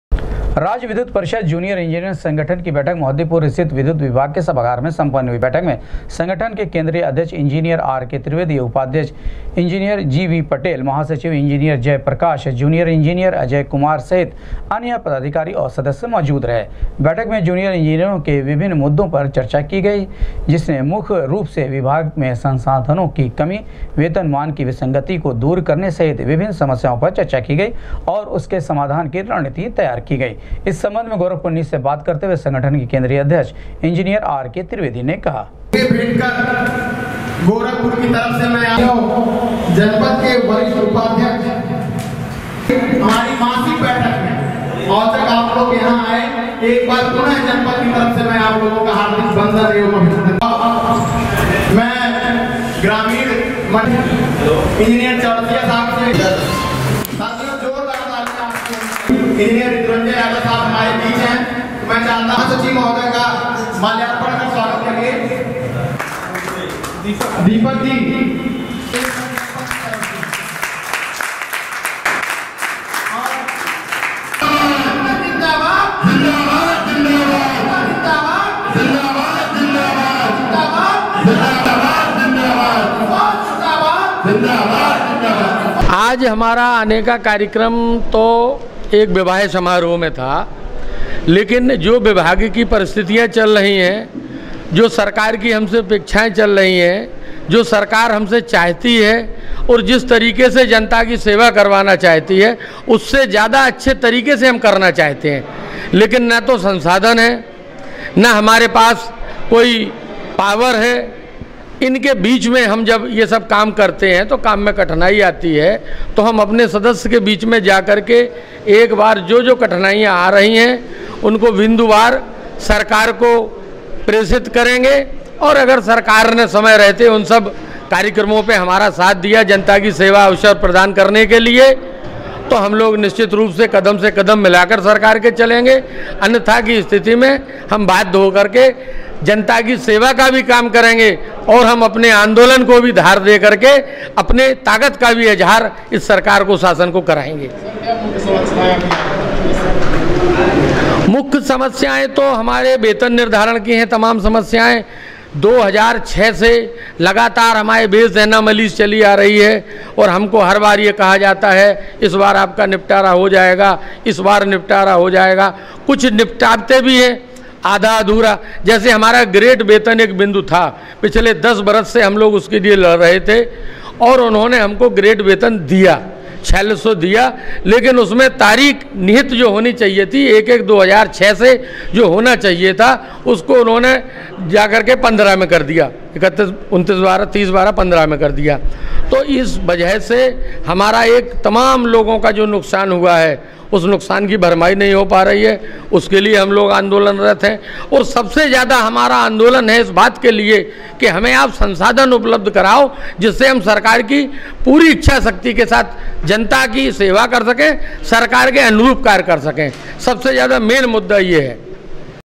राज्य विद्युत परिषद जूनियर इंजीनियर संगठन की बैठक में हुई विभिन्न मुद्दों पर चर्चा, बनाई गई
राज्य विद्युत परिषद जूनियर इंजीनियर संगठन की बैठक में हुई विभिन्न मुद्दों पर चर्चा, बनाई गई समाधान की रणनीति